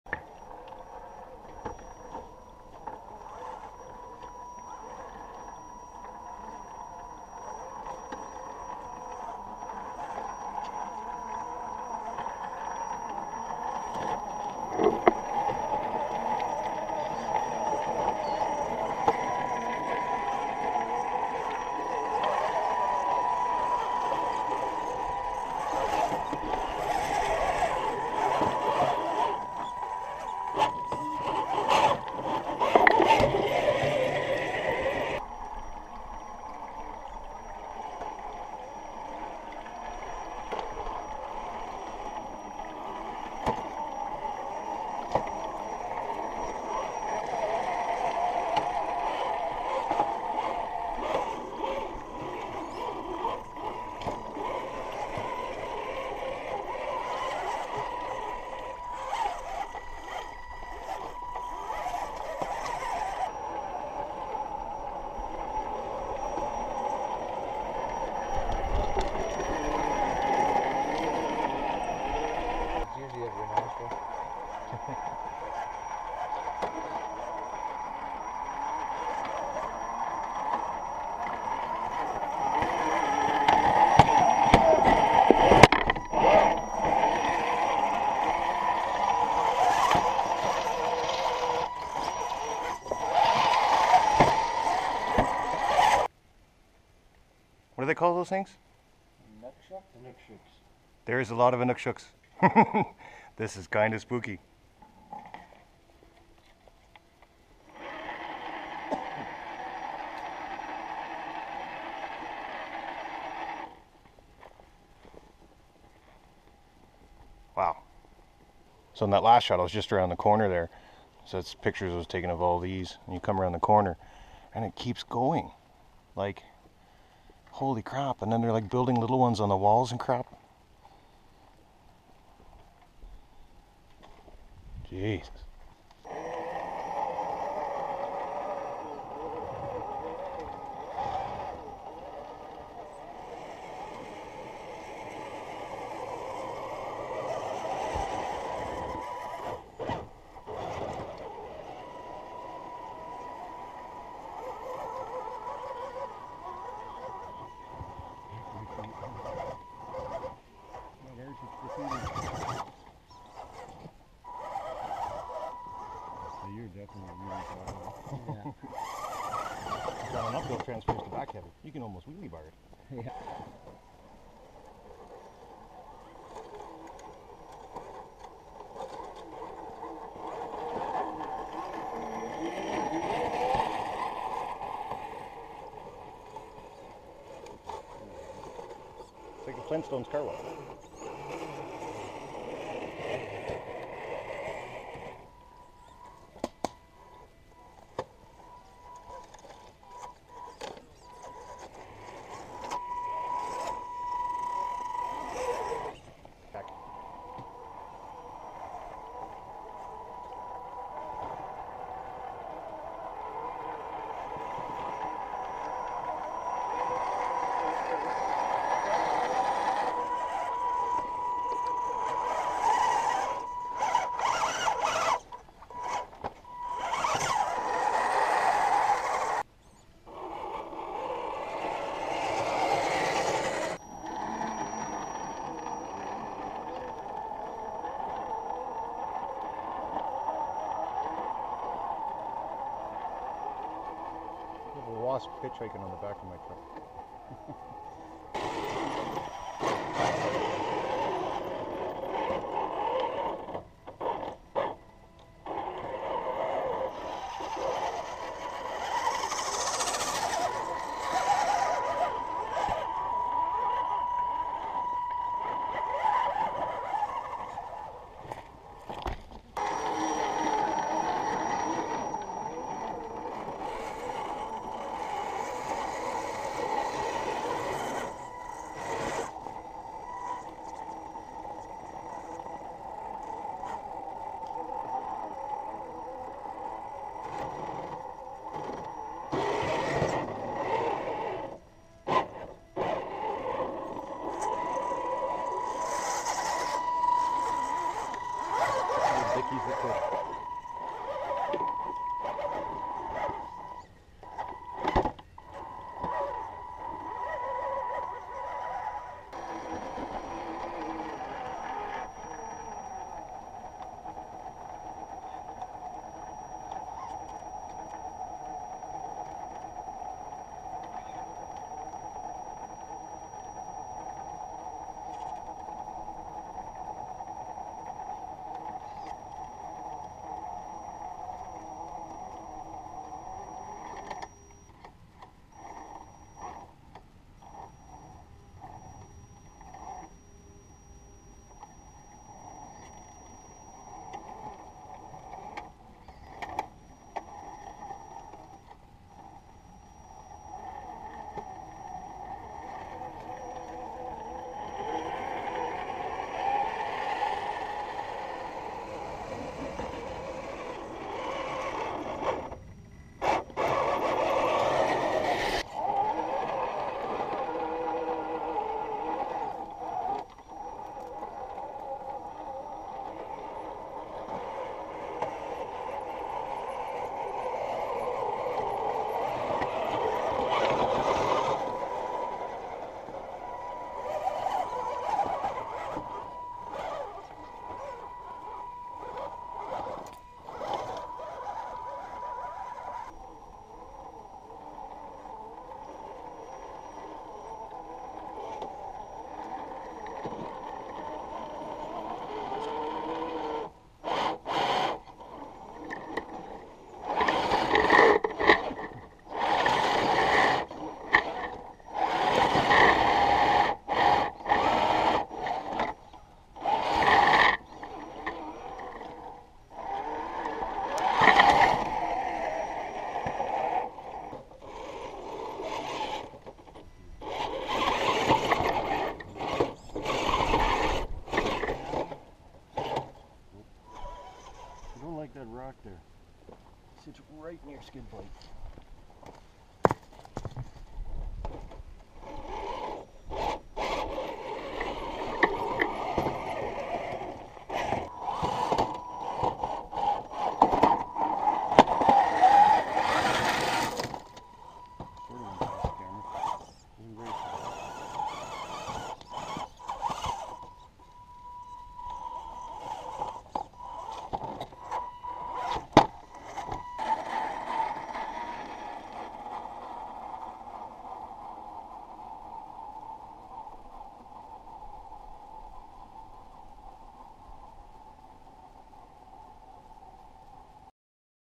RC ATHLETES - Grotto Canyon - Rock Piles Galore!!
Great but weird run lol. Always love going out here. Got a little weirded out though by all the inuksuks. I guess there was a class out there and the teacher was getting them to build these things. There were sooo many lol!!